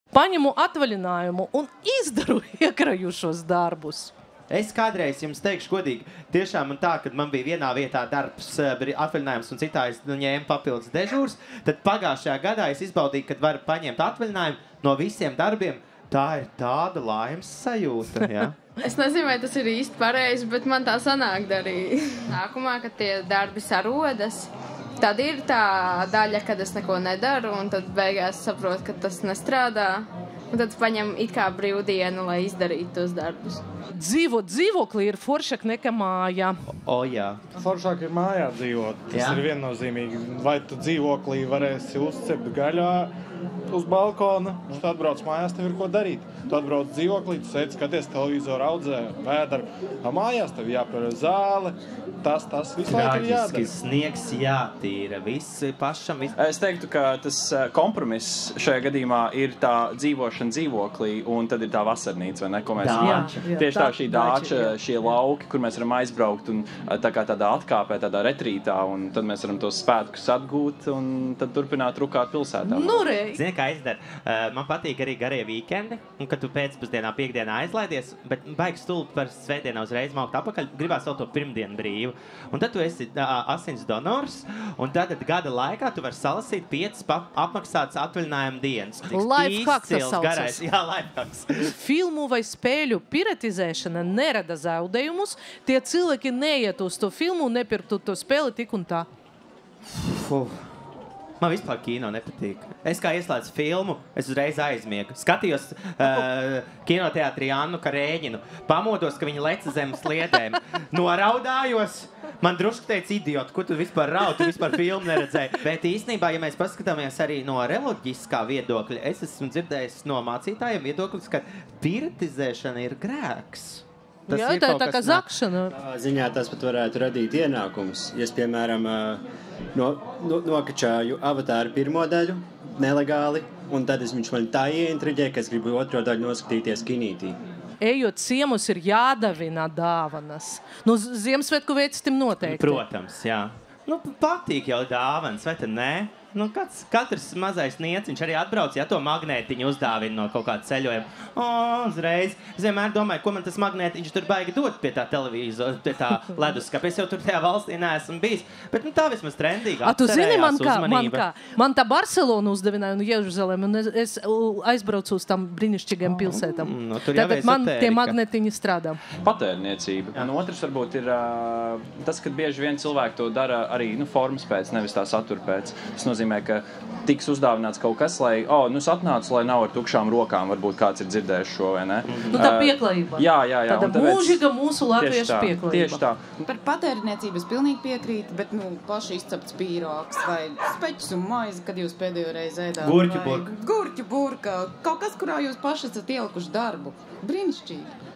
“Pieklājība. Tā mūžīgā mūsu – latviešu – pieklājība.” | Rafaels Ciekurs Nepopulāru viedokļu parādē
Vai ejot ciemos ir jādāvina dāvanas? Saulesbrilles iekštelpās – stilīgi vai galīgi garām? Vai jāpalielina alkohola iegādes vecums un jāierobežo tā tirgošanas laiks? Anna Stroja un Rafaels Ciekurs par nopietniem un pavisam nopietniem cilvēku viedokļiem, kurus esam savākuši Brīvā mikrofona teltij "Radiants" festivālā "LAMPA".